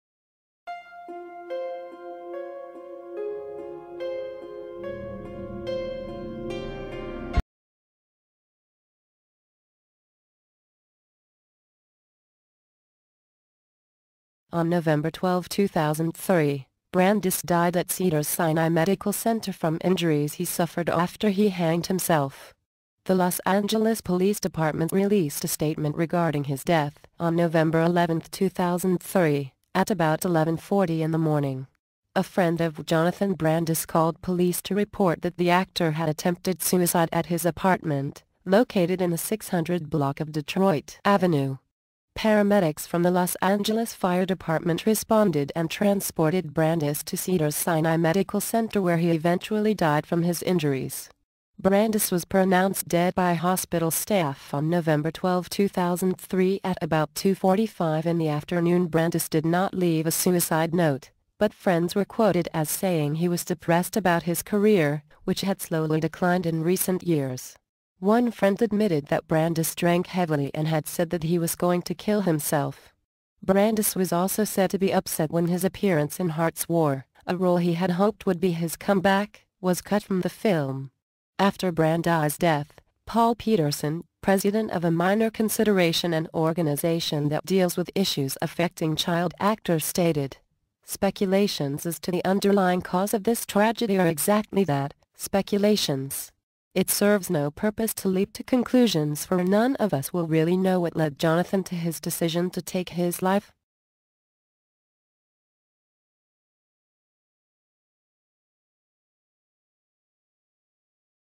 THE DEATH OF JONATHAN BRANDIS
This video details the suicide death of child actor Jonathan Brandis!